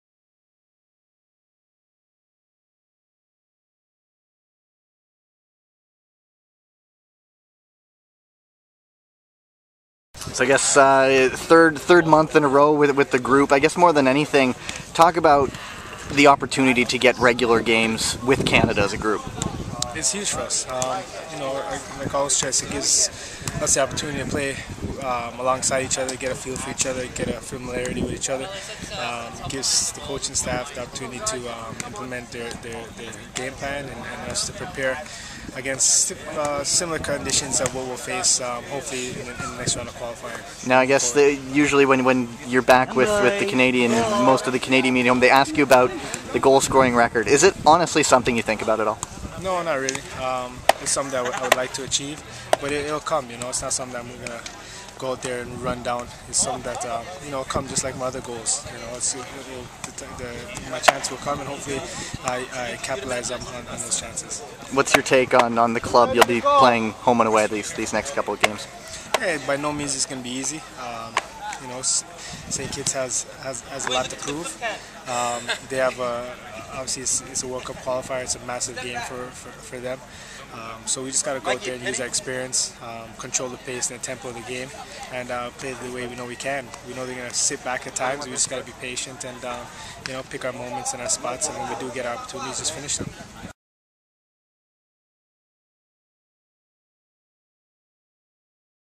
Red Nation Previews St. Kitts-Canada--Dwayne De Rosario
DC United's Dwayne De Rosario is still chasing Dale Mitchell's all-time Canadian goal scoring record into their game against St. Kits. In this interview, DeRo speaks to Red Nation about that and what getting regular games with Canada means to the squad.